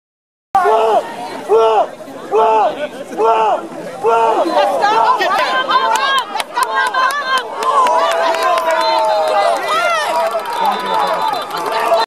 Stepinac's Malcolm Major; nice run
Bias alert: Actually more interested in #40, Tim and his block. this in Stepinac's 49-0 defeat of Monsignor Farrell on November 1. No touchdown on this though; the play was called halfway back because of a holding penalty downfield.